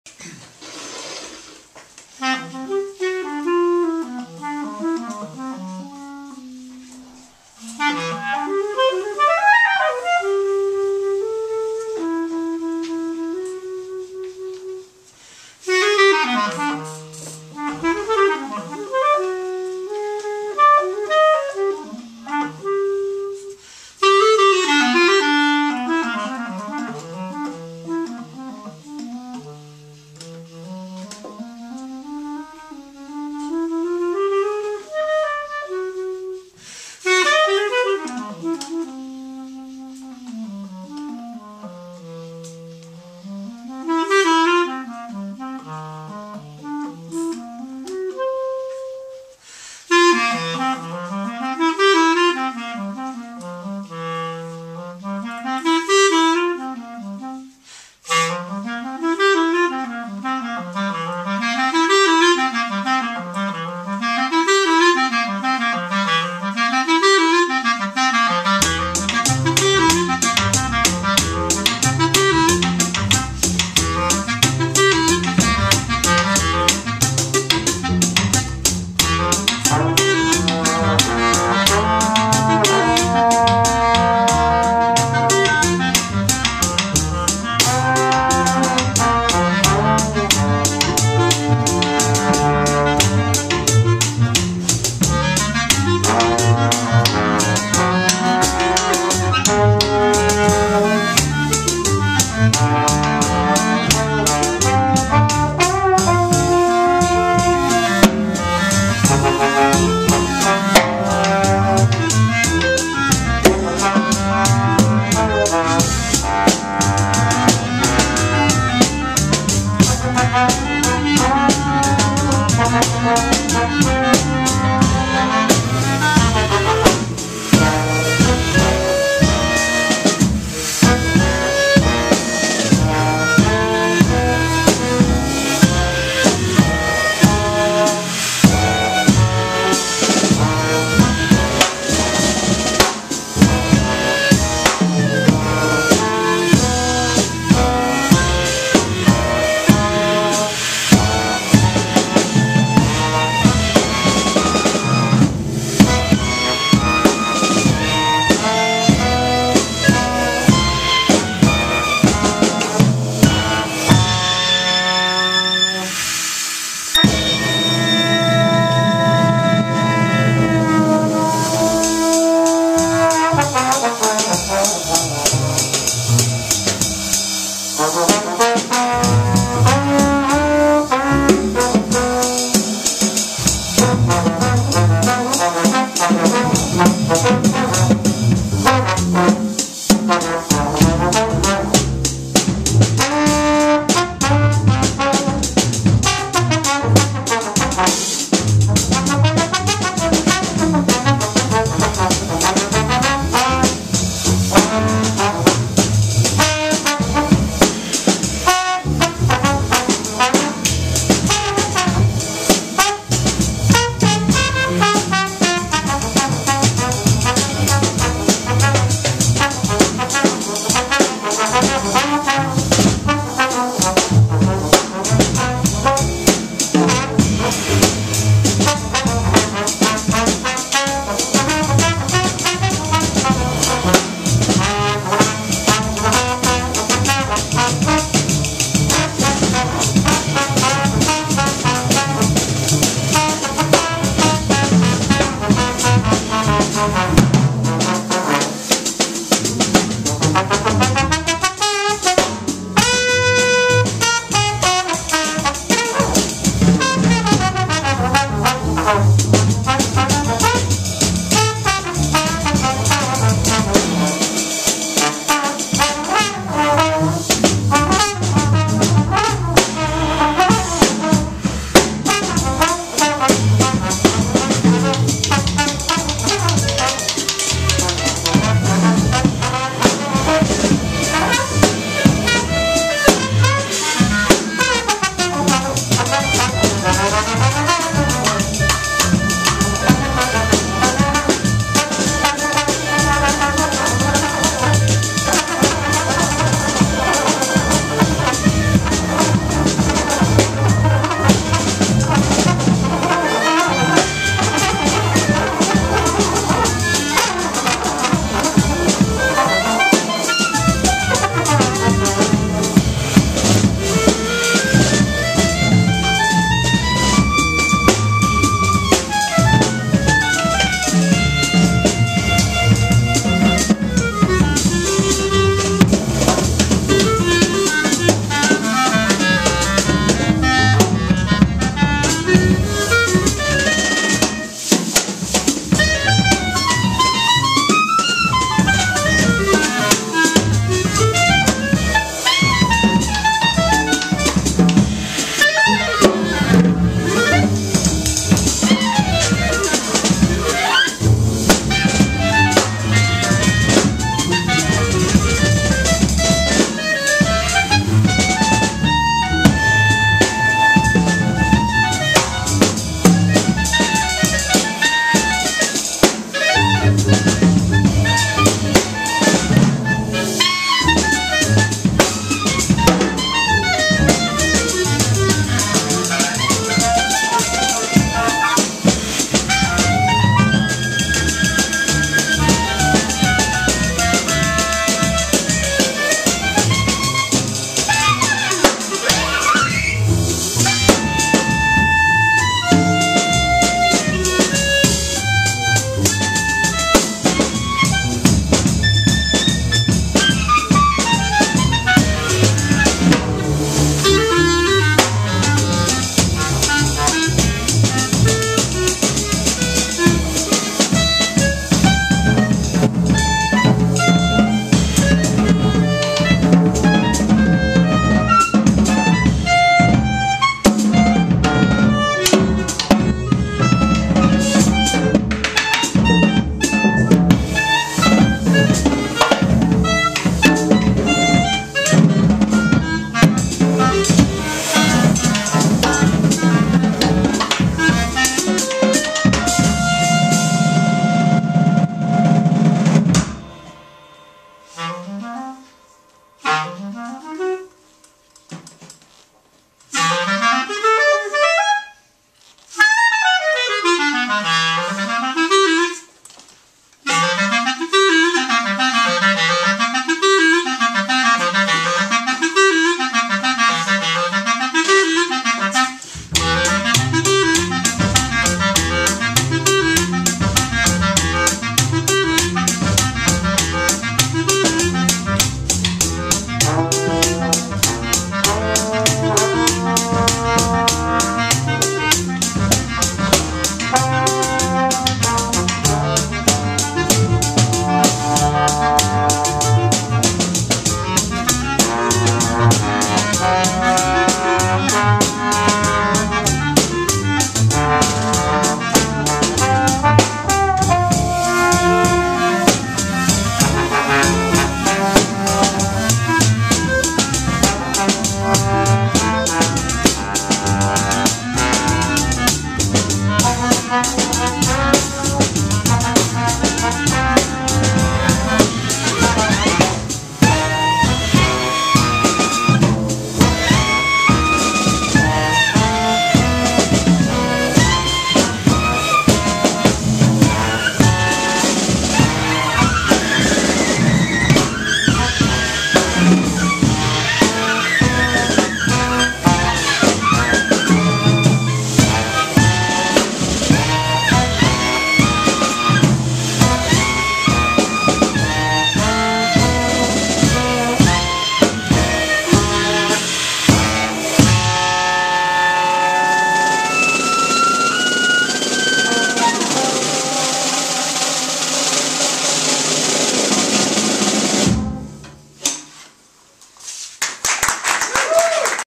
James Falzone's KLANG @ I-Beam, 3-15-13 2/4
James Falzone -- clarinet
Jeb Bishop -- trombone
Jason Roebke -- bass
Tim Daisy -- drums